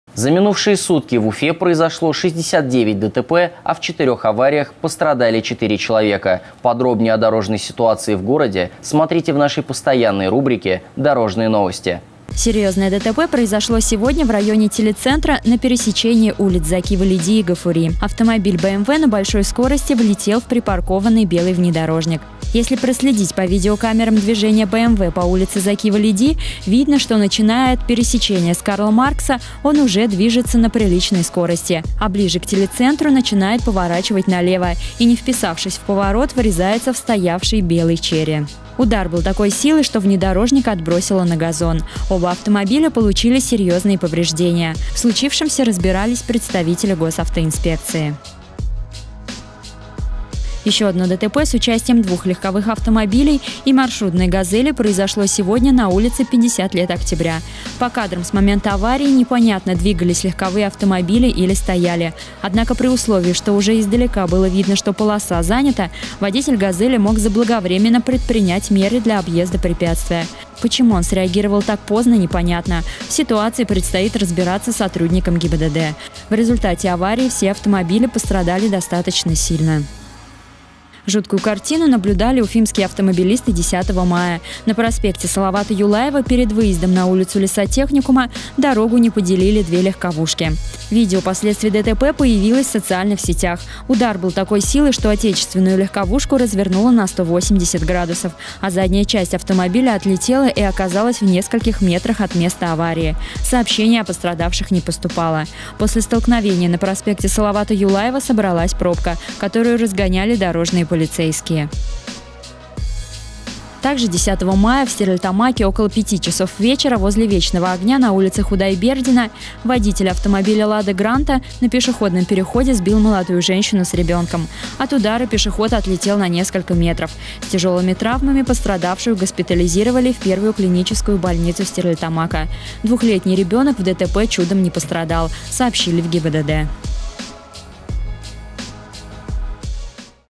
Дорожные новости 12.05.16
За минувшие сутки в Уфе произошло 69 ДТП, в 4 авариях пострадали 4 человека, подробнее о дорожной ситуации в городе смотрите в нашей постоянной рубрике Дорожные новости.